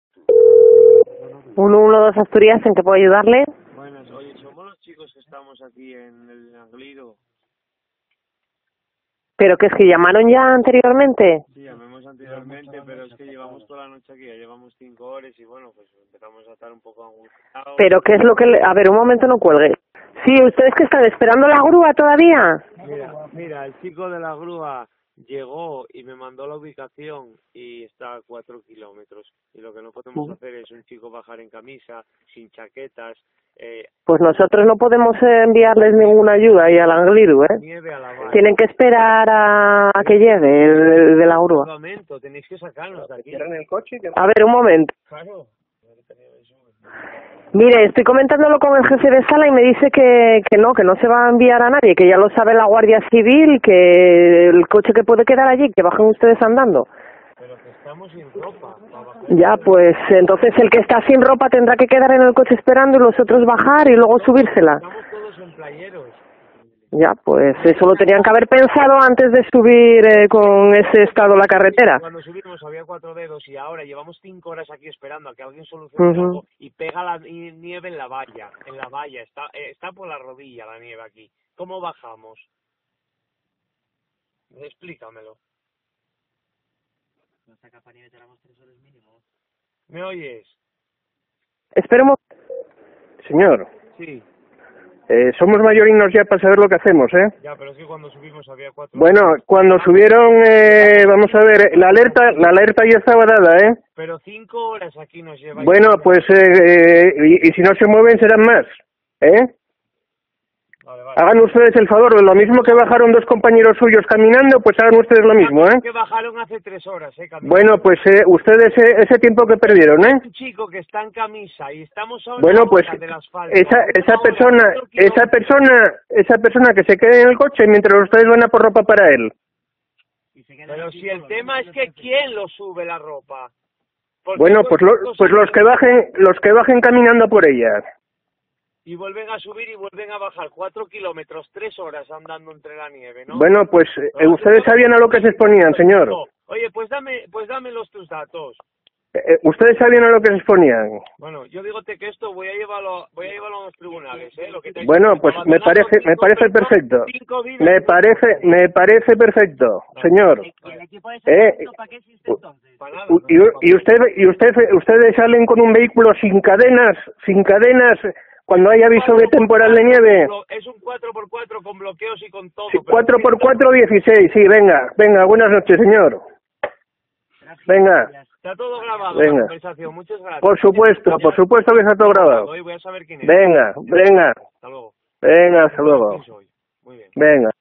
“Ya somos mayorinos”: la respuesta del 112 a los veinteañeros atrapados en un puerto asturiano
Siete veinteañeros, en chándal y playeros, quedaron atascados en el Angliru dentro de un todoterreno. LA NUEVA ESPAÑA